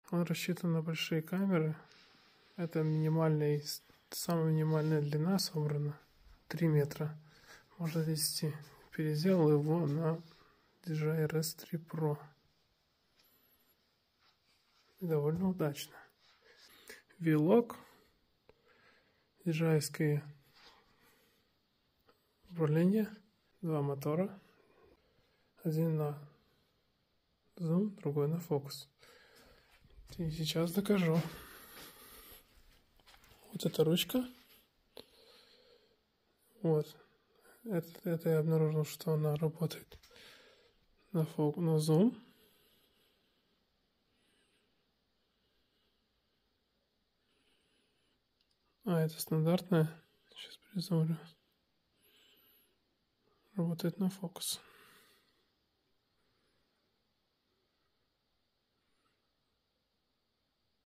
Dji rs3 pro with TWO MOTORS ?? NO LIDAR. Jimmy jib crane
I build custom Jimmy jib with Dji rs3 pro head and two dji motors, one for zoom other for focus.